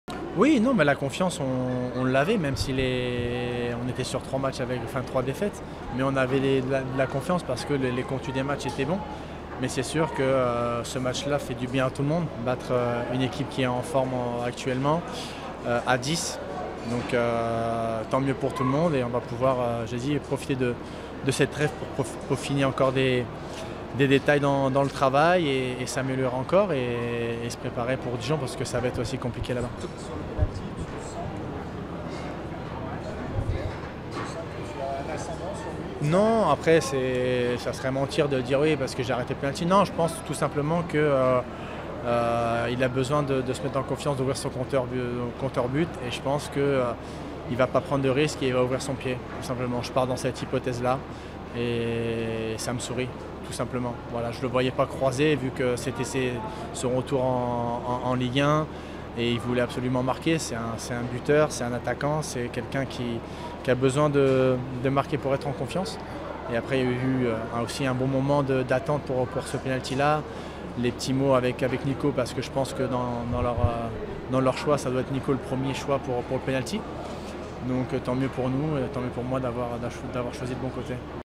Butelle «Ce match fait du bien» - Foot - L1 - Angers
Après trois défaites lors des trois premiers matches de la saison, le gardien d'Angers était soulagé après la victoire (1-0) face à Lille.